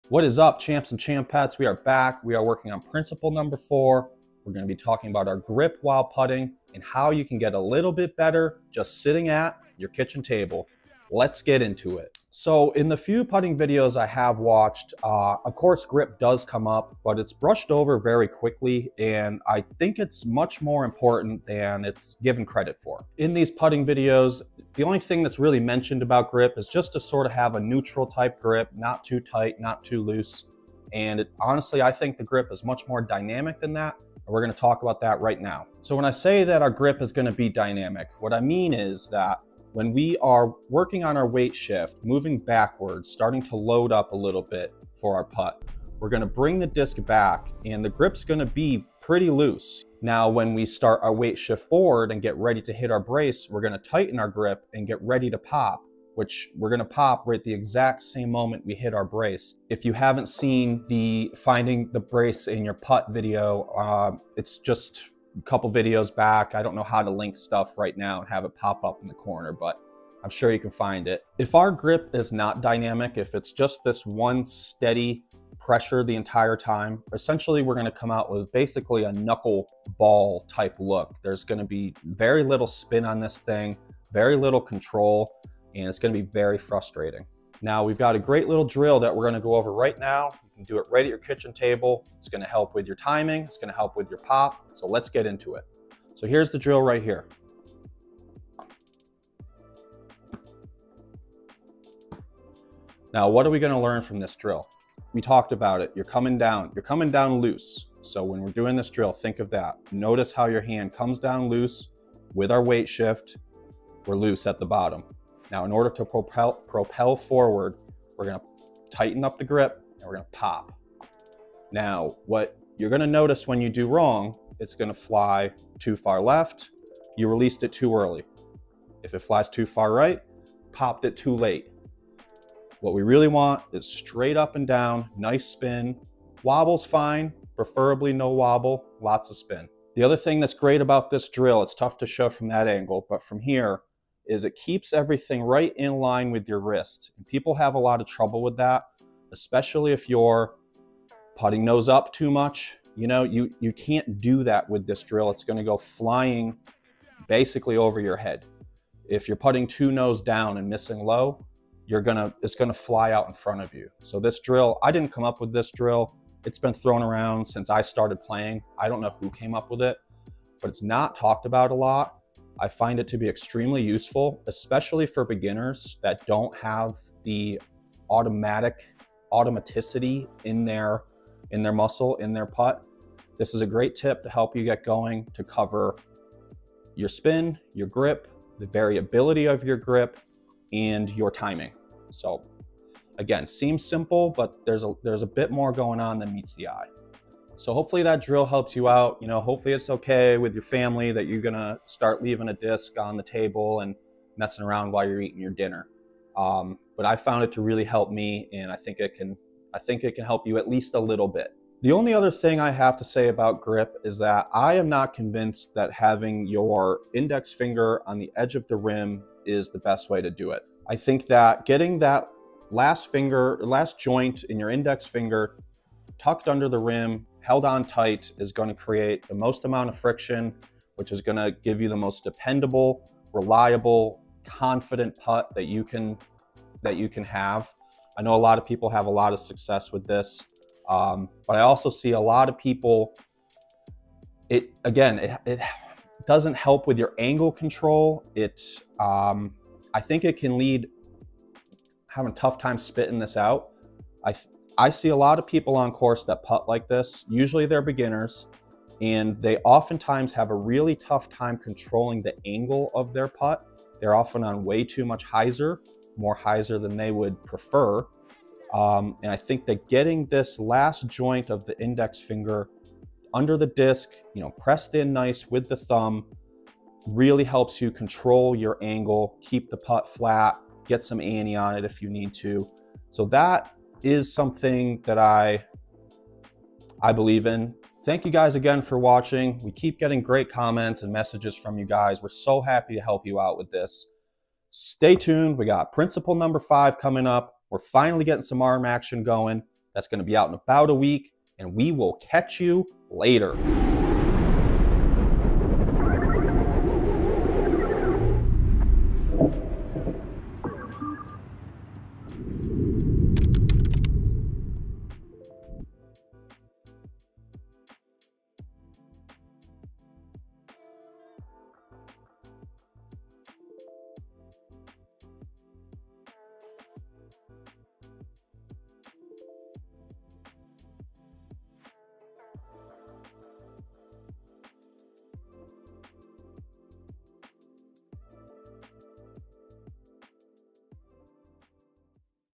Get a Grip on Your Putt - Advanced Putting Grip Tips + Drill - Principle #4
We are working on the 10 principles of the PuttConfidently Approach, and we have to make sure we’re getting each step right along the way. Otherwise, we will not have the fundamentals in place that will allow us to really grow in our game. This is Principle #4

Understanding how grip affects your putt is a huge part of making 20 footers more consistently. This video goes over what we think about grip, and why we believe it deserves more thought than other putting top videos give grip. We also will be highlighting a drill that we find very helpful, and our thoughts on index finger placement.

To check out a ChampCap Solo which was featured in this video go This product will really help you lock in the fundamentals we are talking about in the PuttConfidently Approach, although you do not need one to grow your game this year. - Kevin